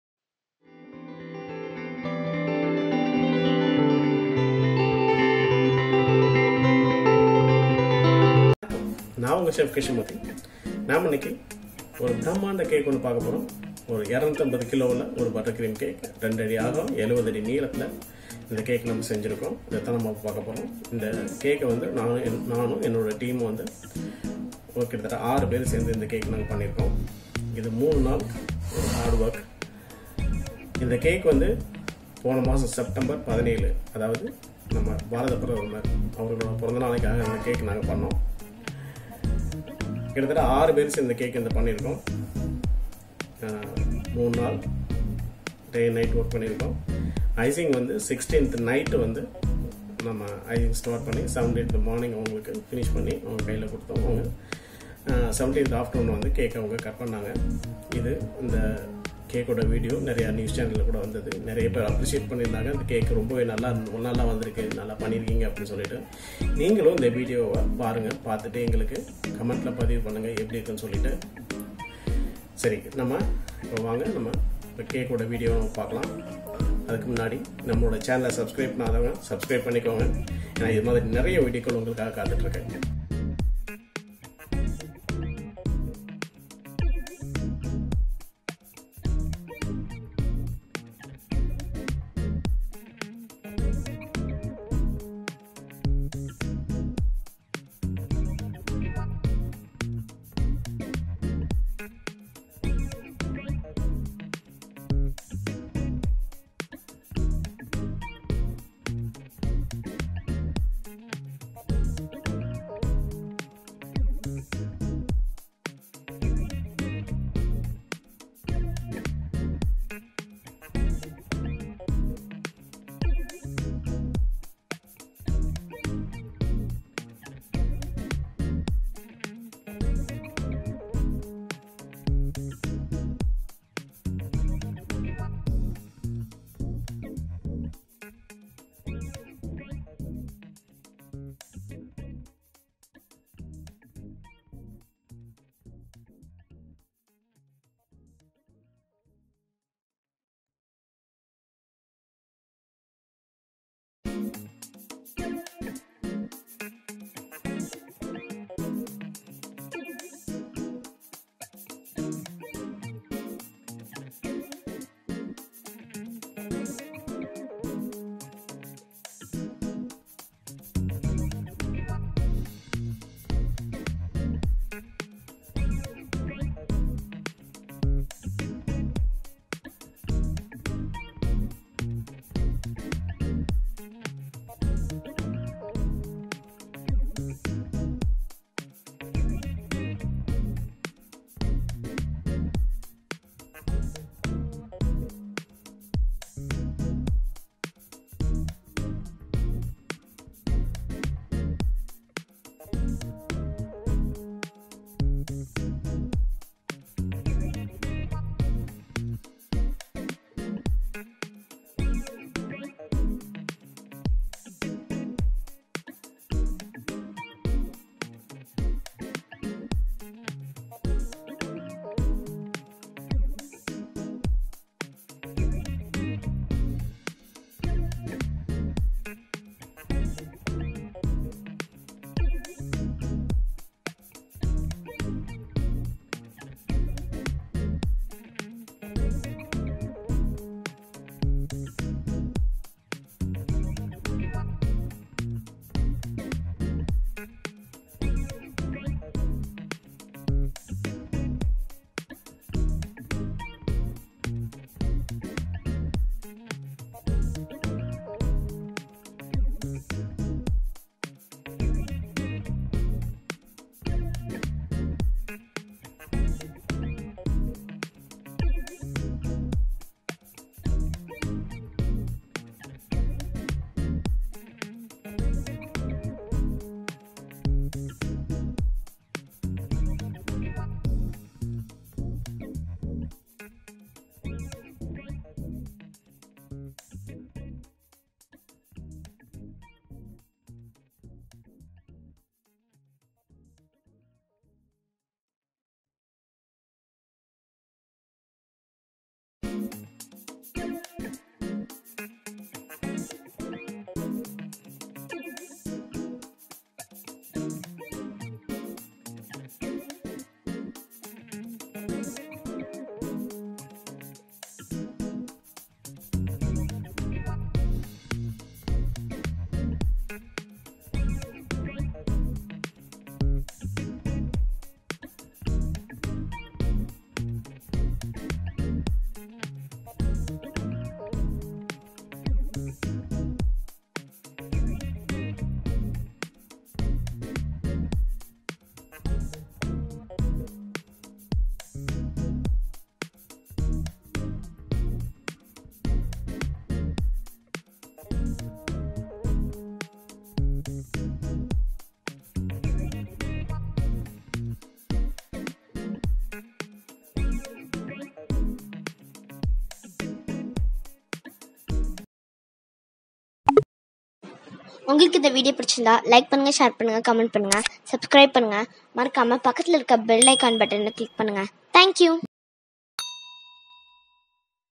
# CELEBRATION CAKE FOR OUR BELOVED PM MODI JI # பிரதமர் பிறந்த நாள் கேக் பிரமாண்டமாய்...# B'DAY CAKE
Hello friends,
I have made a hilarious cake for our beloved PM Modi Ji which i have enjoyed making cake actually my passion is being a pastry designer so i loved to make this cake even though there is no profit on this cake.

my team members who are worked with me have given a solid effort on making this cake. Most of the people were asked me how this is happened i said this is all because of my team.

Now I have started receiving cake orders from my friends and neighbours also receiving orders from customers through word of mouth.

we are experiencing difficulties at this period which no one would experienced before and most of the people were unemployed i hope this will change soon. I pray the god not to retaliate again such disaster world wide.

Say we do not lose our hope and keep strive towards our life.

Please watch the video and make golden comments in the greatest comment box.

Thank you for your support and keep support us.